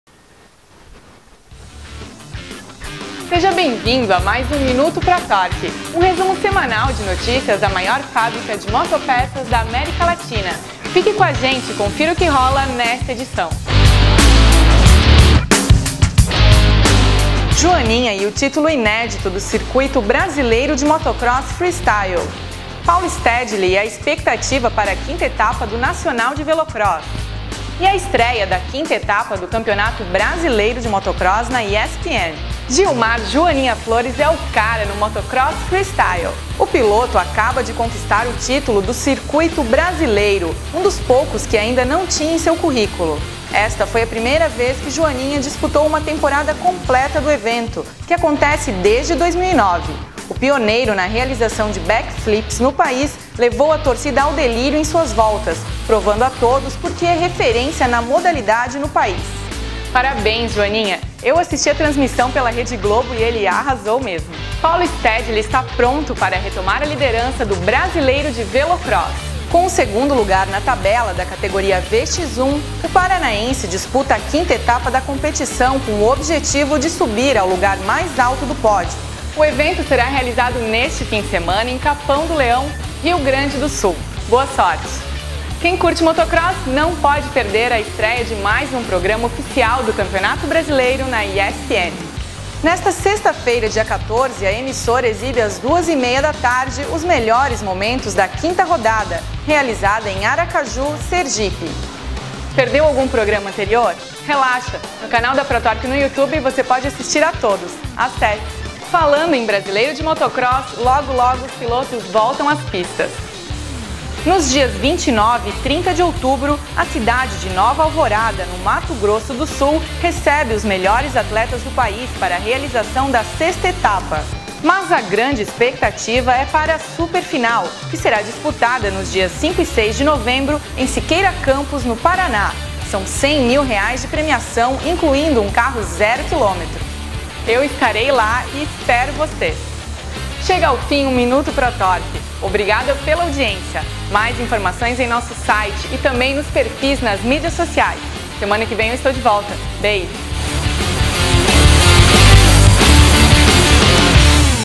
Minuto Pro Tork - Edição 29
O Minuto Pro Tork, um resumo semanal de notícias em vídeo da maior fábrica de motopeças da América Latina, está cheio de novidades. Nesta edição você confere a conquista inédita de Joaninha do Circuito Brasileiro de Motocross Freestyle, a expectativa de Paulo Stedile para a quinta etapa do Nacional de Velocross e a estréia da quinta etapa do Campeonato Brasileiro de Motocross na ESPN. Assista agora e aproveite para mandar sugestões!